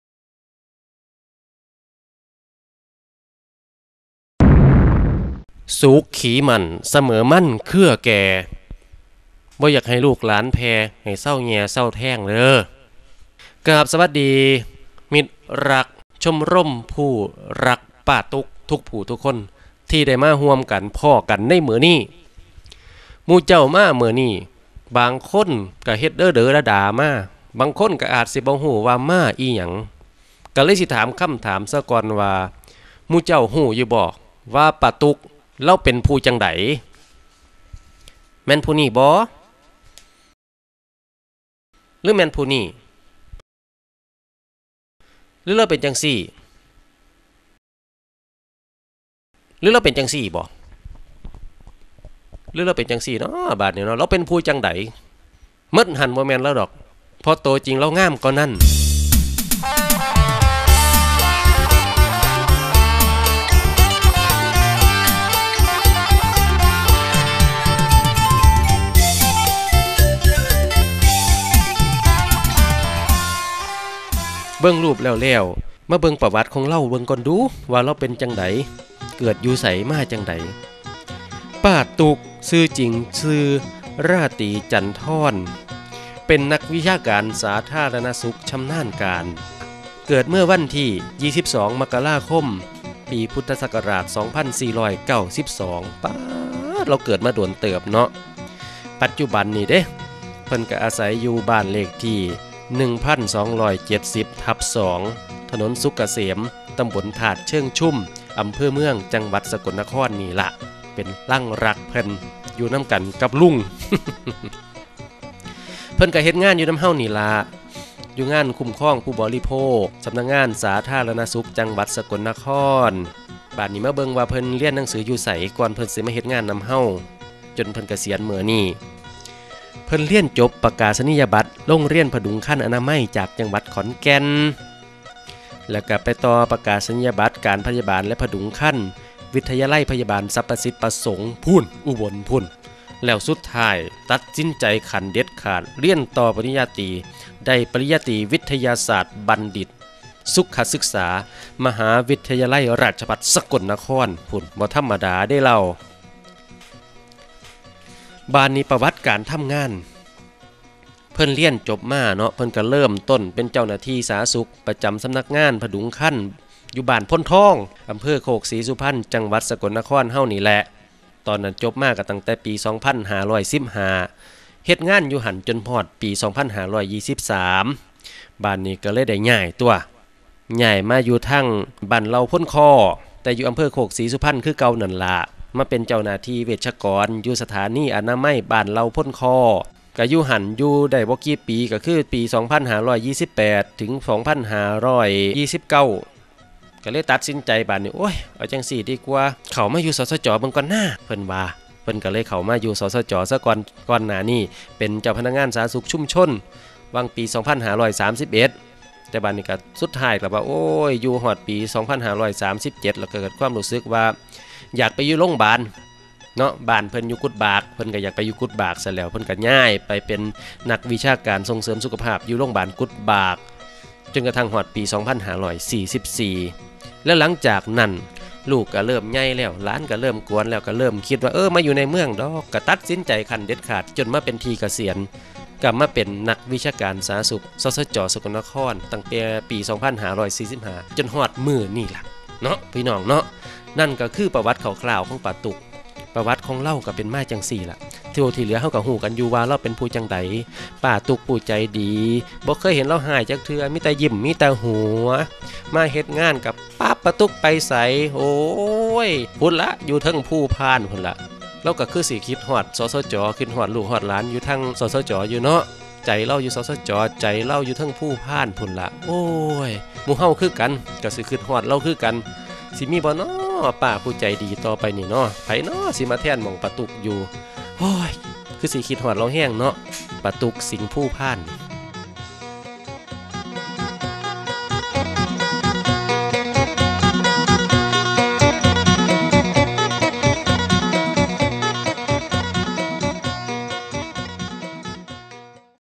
ราตรีนี้ อีกยาวไกล.....รักนะป้าตุ๊ก
ประวัติชีวิต และการทำงานของป้าตุ๊กราตรี จันทร
นักวิชาการสาธารณสุขชำนาญการ
กลุ่มงานคุ้มครองผู้บริโภค
สำนักงานสาธารณสุขจังหวัดสกลนคร
9 ตุลาคม 2552